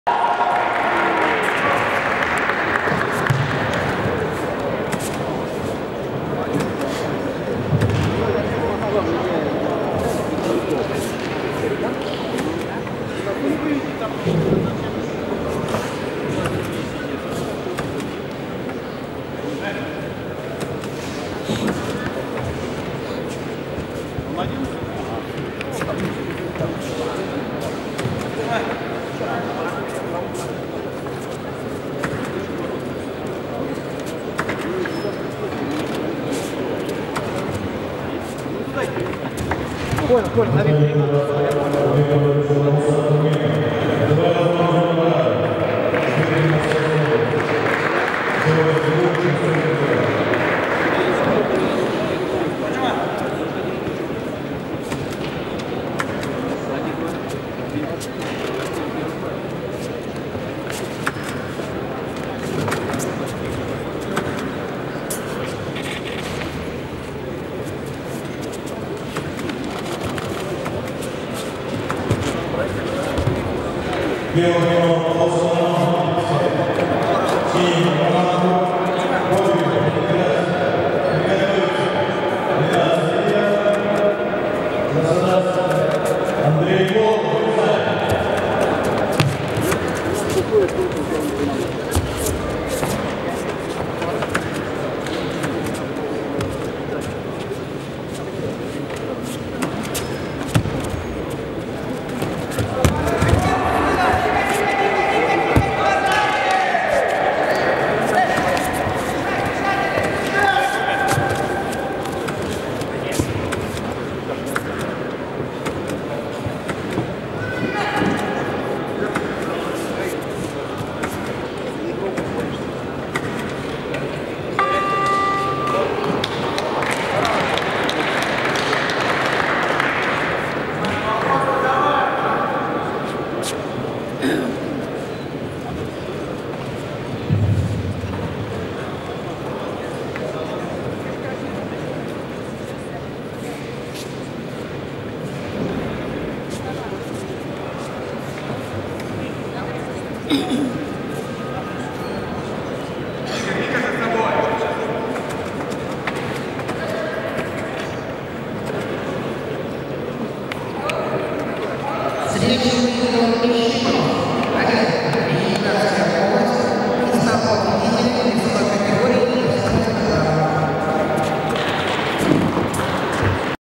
2019 дзюдо финал -100 ИШИМОВ - АДАМЯН Чемпионат МВД РФ Рязань judo
ИШИМОВ Олег - АДАМЯН Армен